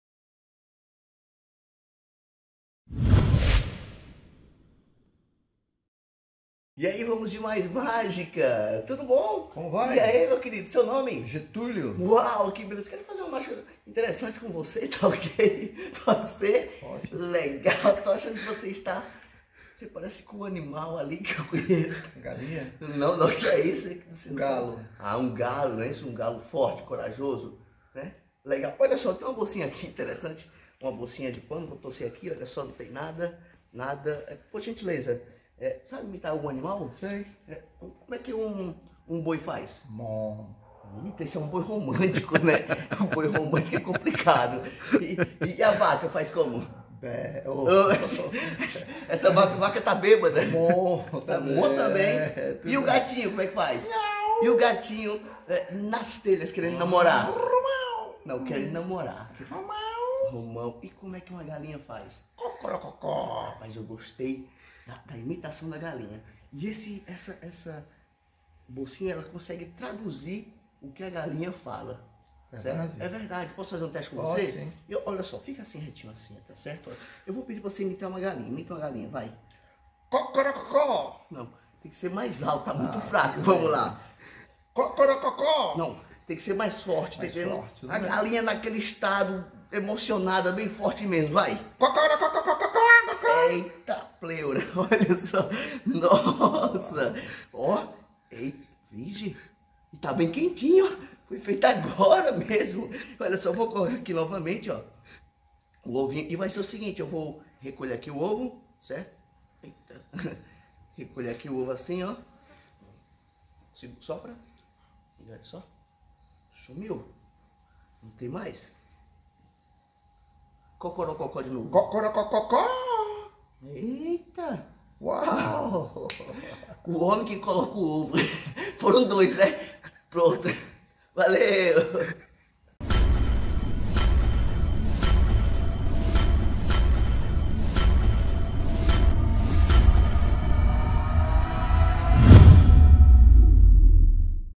SACO DO OVO
Um efeito cômico, muito bom para shows de close-up e palco! Todos vão amar esse efeito e dar muita risada!😂🎩

Sucesso garantido! Você encontra esse e outros produtos incríveis na @magicamais ou em nosso